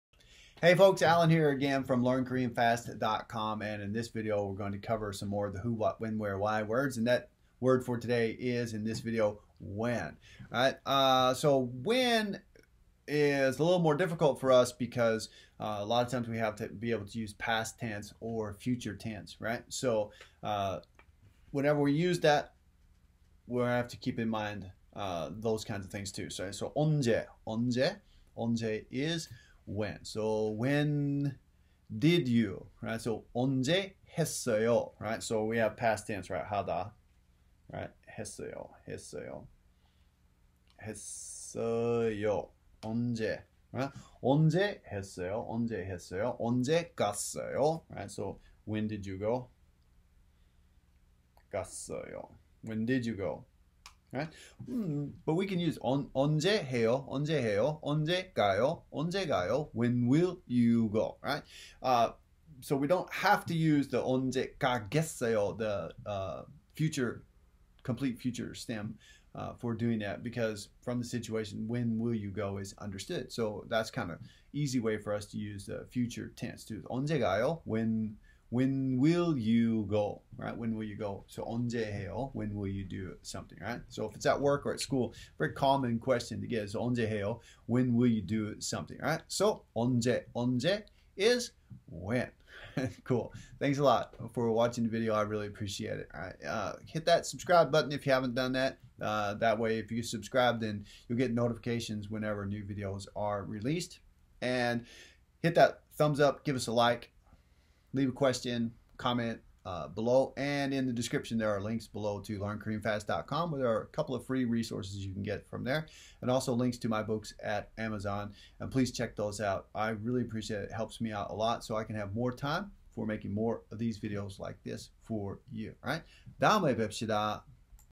Learn Korean Fast - how to say when in Korean
Learn how to say "when" in Korean. It is important to get those 5 w words down. Here's a key one with "when".